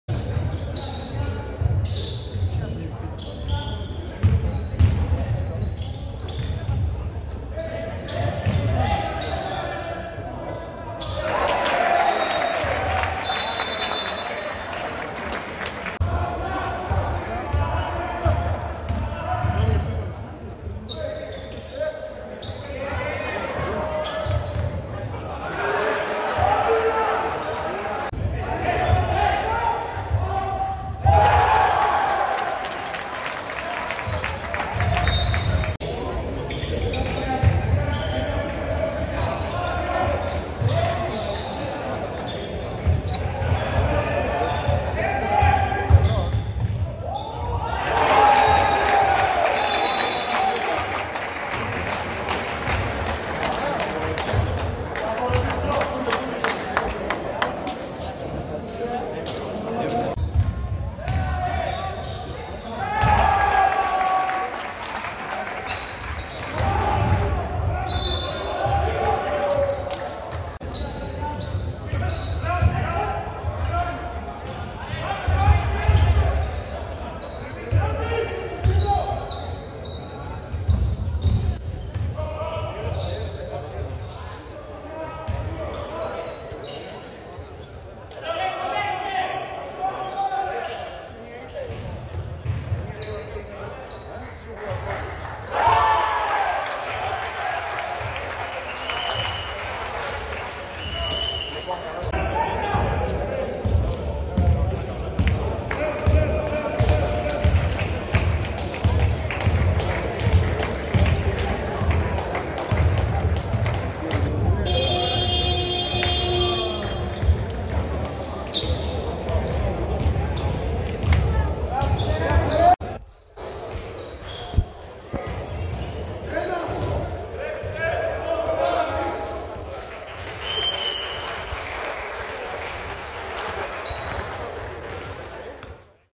2018-19 Α.Ε. Διόσκουροι - Αργος Ορεστικό 61-49
Πρωτάθλημα Α' Κατηγορίας ΕΚΑΣΔΥΜ 2018-19
2ος γύρος, 7η αγωνιστική, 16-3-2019
Α.Ε. ΔΙΟΣΚΟΥΡΟΙ ΚΟΖΑΝΗΣ - ΑΡΓΟΣ ΟΡΕΣΤΙΚΟ 61-49